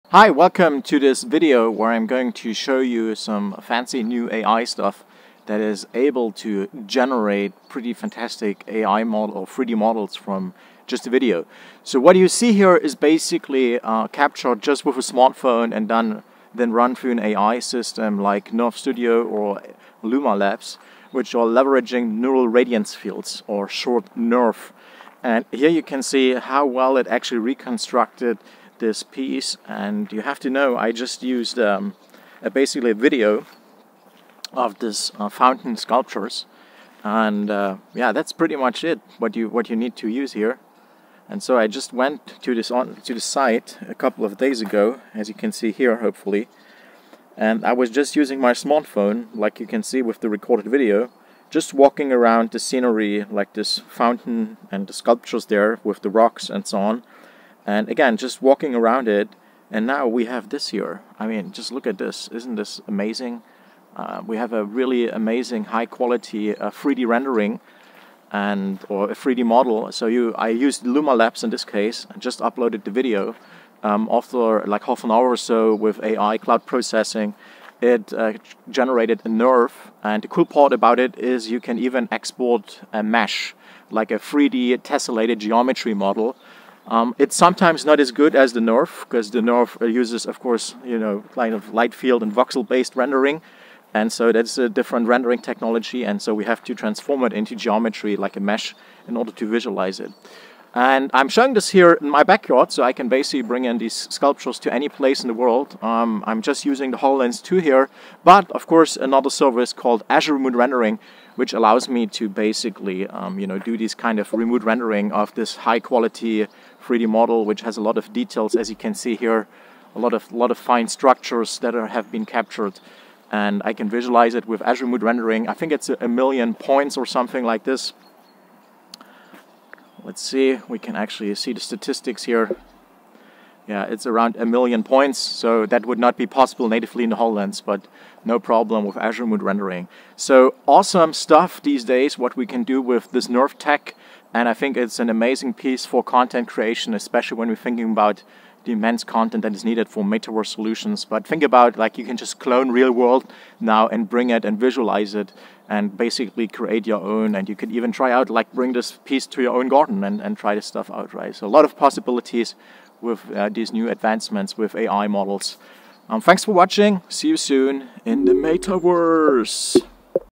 Mobile Phone Video ▶ AI NeRF 3D ▶ AR HoloLens 2 🚀
This HoloLens 2 Azure Remote Rendering demo is visualizing a 3D model that was created simply from a mobile phone video recording leveraging AI-based Neural Radiance Fields (NeRF). 📱

NeRFs are becoming a game changer for 3D content creation. 🤩

Remote Rendering visualizes huge 3D models with hundreds of millions of polygons or points remotely on Azure GPU VMs and streams in real-time to the HoloLens 2 which could otherwise only render less a small fraction of this large scanned model. ARR works without perceptible delay and with interactions! 🤯

Note that videos captured on the device unfortunately lack the hardware late stage reprojection and therefore appear much choppier and drifting than what the user is actually seeing in true stereo 3D. Also the MRC video recording is using some weird blending with the background (there is actually nice color) and is just recording at 30 fps although it's actually rendering at 60 fps. 🤷‍